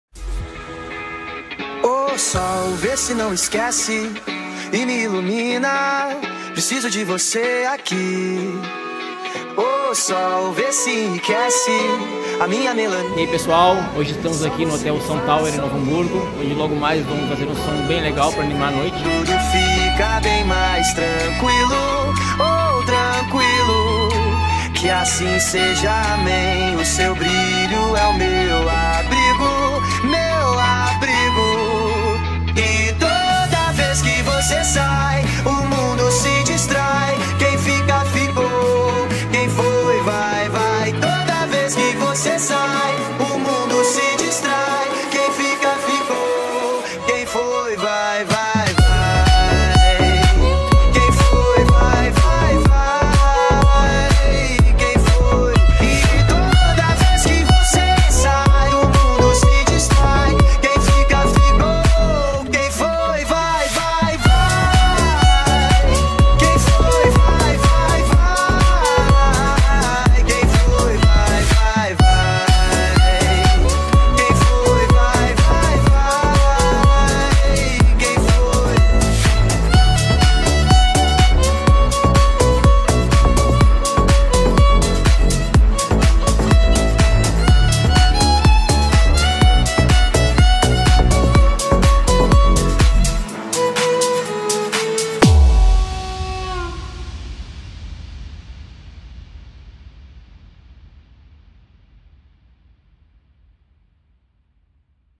DJ e Violino - Show na Pista | DC PROJECT LIVE
Show realizado em aniversário no Hotel Swan Tower em Novo Hamburgo, RS

Produção do video: Bruno Vargas (@bvargasfilmaker)